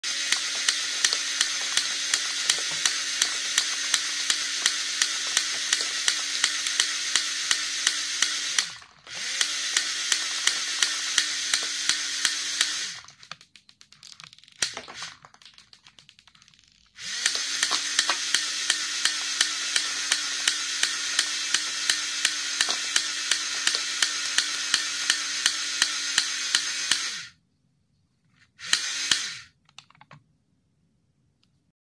ミツワモデル リモコンB・B戦車シリーズ ガンランチャーG-3・主力戦車 砲撃（MitsuwaModel RemoteControlTank Main Battle Tank Artillery）
ミツワモデル リモコンB・B戦車シリーズ ガンランチャー G-3・主力戦車 の 砲撃 の様子。

フルオート射撃 機構
装弾数 60発
毎分140発
単2電池 3本使用

Mitsuwa-Model Remote Control B-B Tank Series Gun Launcher G-3 and main tank shelling scene.

Fully automatic firing mechanism
60 rounds loaded
140 rounds per minute
Uses 3 AA batteries